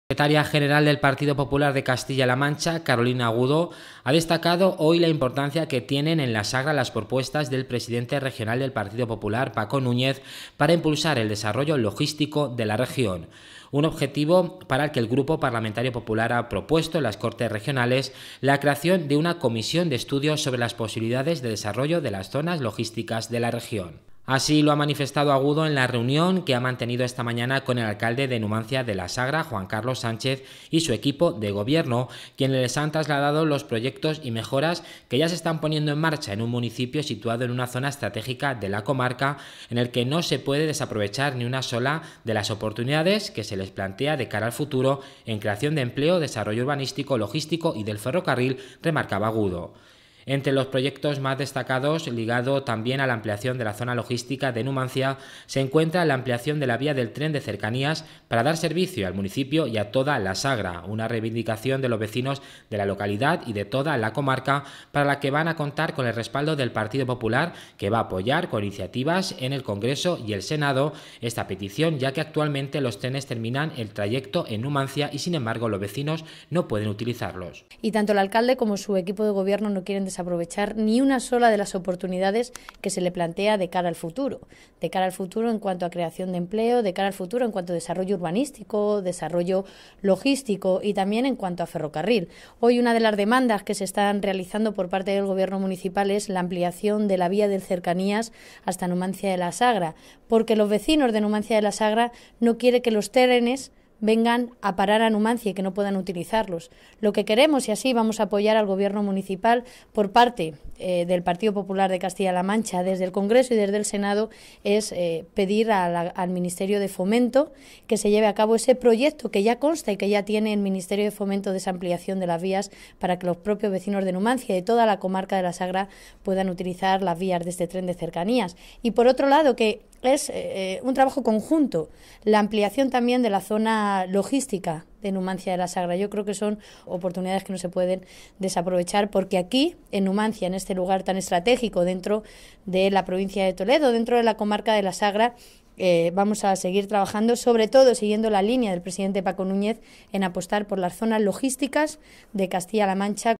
Agudo destaca la importancia que tienen para La Sagra las propuestas
Agudo destaca la importancia que tienen para La Sagra las propuestas de Núñez para impulsar el desarrollo logístico de la región